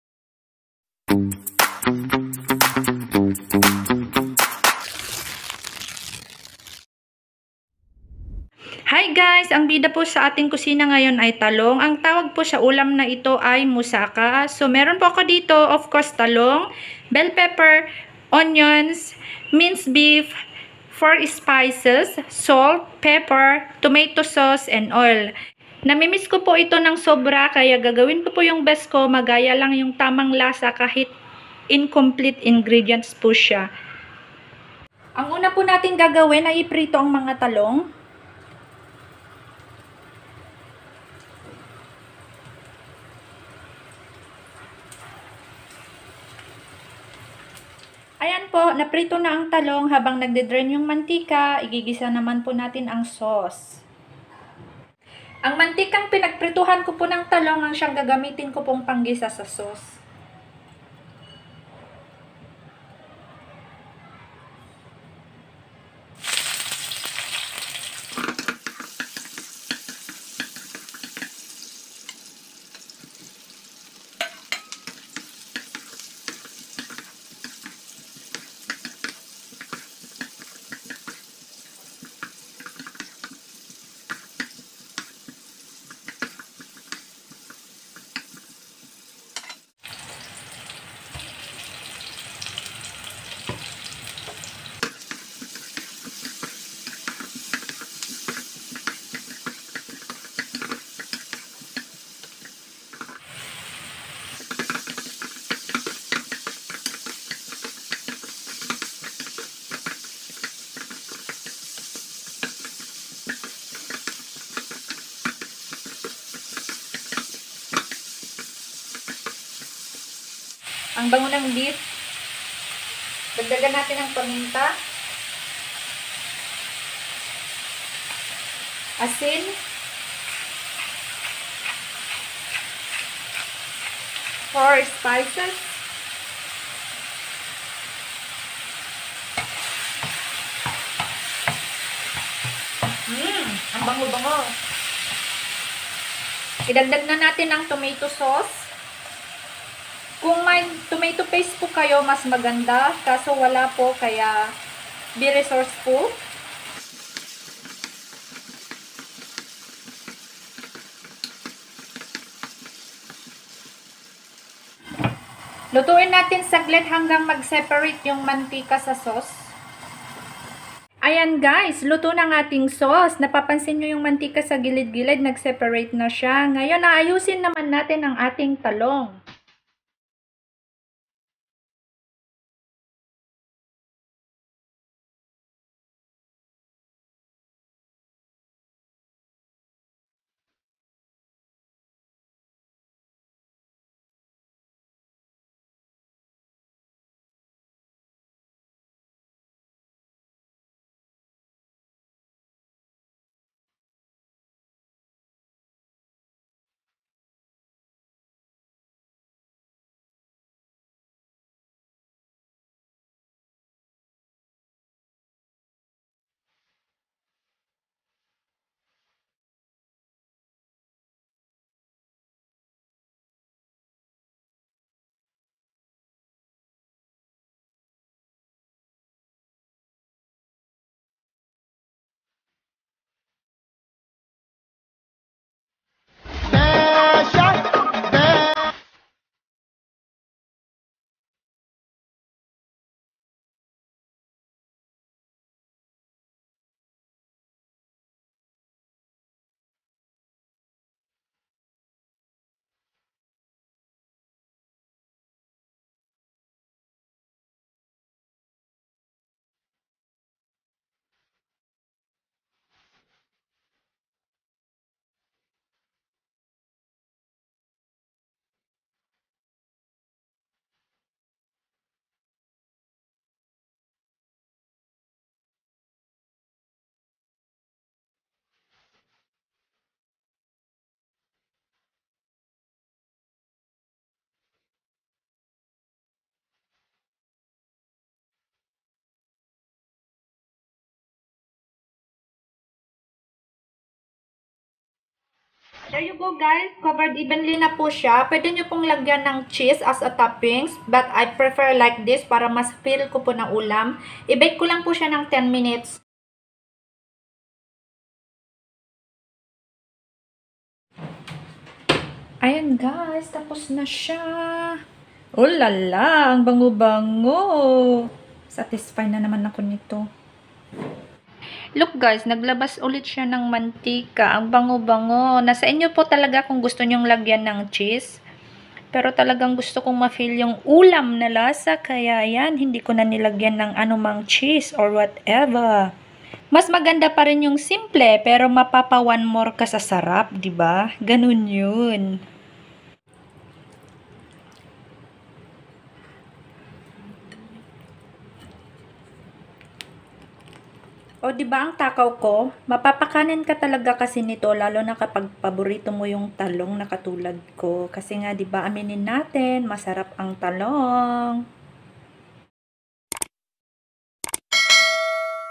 MOUSSAKA
This dish is similar to LASAGNA but as a pure blooded Filipino I didn't put any cheese or bechamel on it because I want to eat with rice as viands🍛🍚☺️

MY INCOMPLETE INGREDIENTS
- onions
- bell peppers (any color)
- minced beef (chicken or pork)
- salt & pepper
- four spices
- oil
-tomato sauce (paste)

Moussaka (/muːˈsɑːkə/, UK also /ˌmuːsəˈkɑː/, US also /ˌmuːsɑːˈkɑː/) is an eggplant- or potato-based dish, often including ground meat, which is common in Egypt, the Balkans and the Middle East, with many local and regional variations.